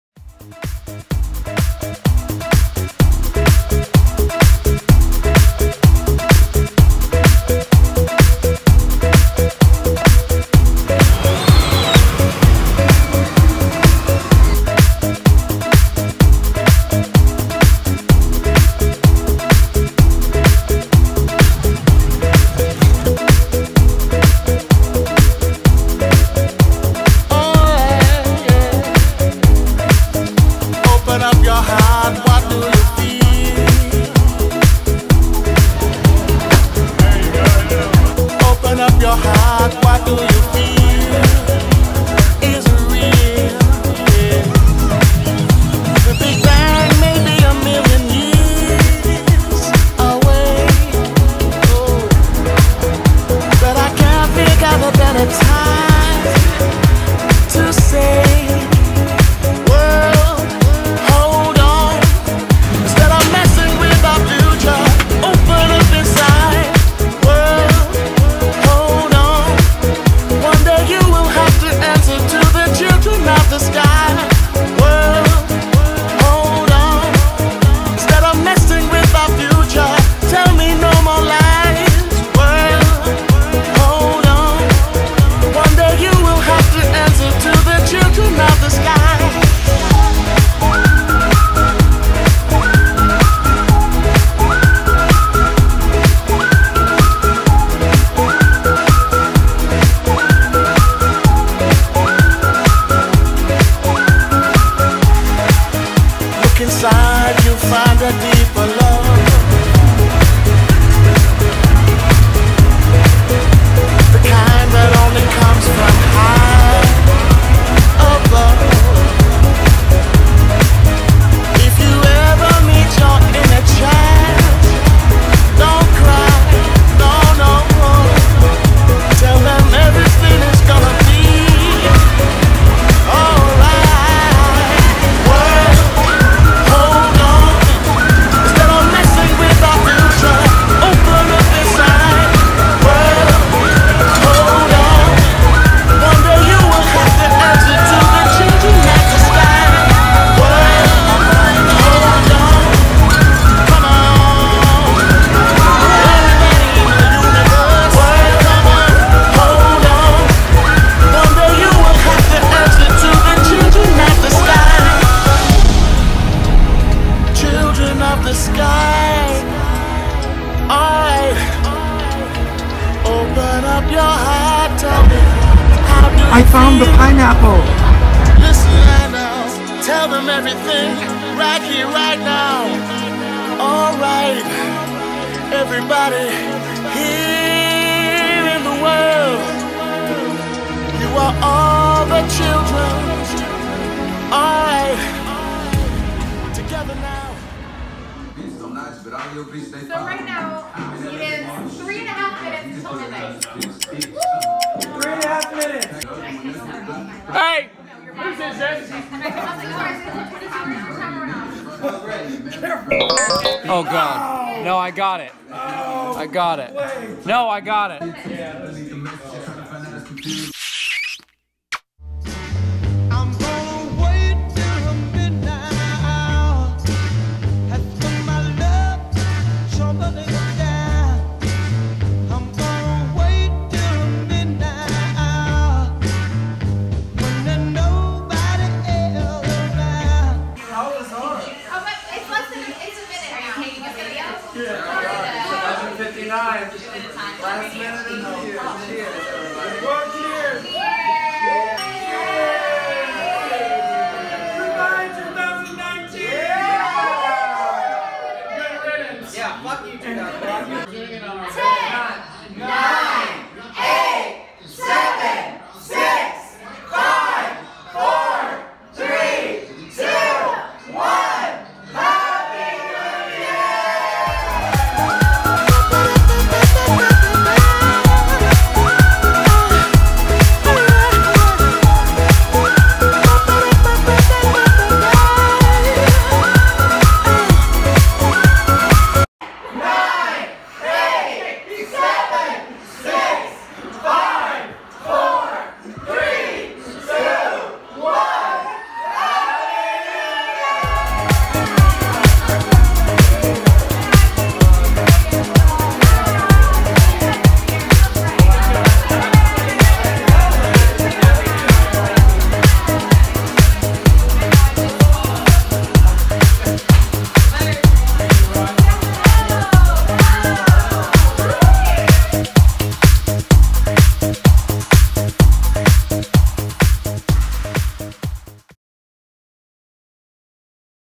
Sarasota Carnival/New Year's Countdown/2020!
Walked around the carnival downtown, then biked to join the Bright Star clan for the 2020 countdown--  Happy New Year!